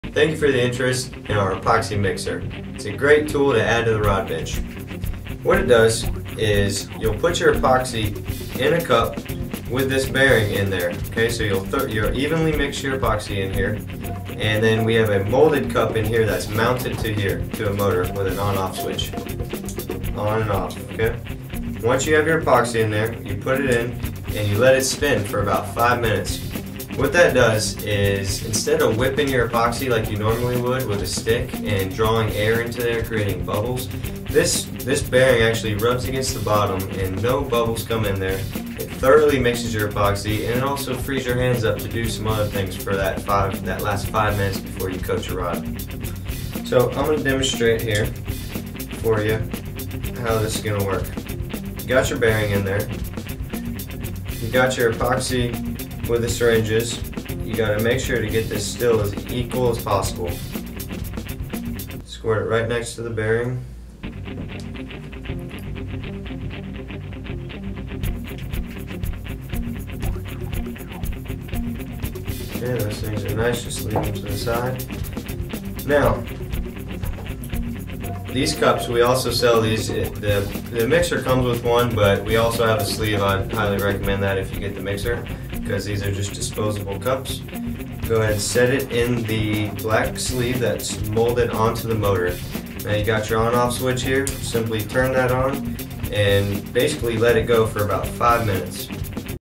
Automatic Epoxy Finish Mixer: Mud Hole Exclusive Look
This new tool from Pacific Bay is designed to mix multi-part finish and epoxies with perfect no-bubble results, and is driven by it's included 24 RPM A/C-powered motor.

Key Features:

    • Mixes rod finish in 3 to 5 minutes - without bubbles
    • Solid stainless steel "bearing mixer" insures thorough mixing of A & B resins
    • Sturdy Base - Rugged construction will provide years of service
    • Reliable On/Off switch is integrated into the 6' extension cord
    • Includes 3 disposable mixing cups to get you started
    • Works with standard 2.5oz mixing cups sold here
    • 24 RPM - AC 120V 50/60 Hz 3.8W - 220V available for international use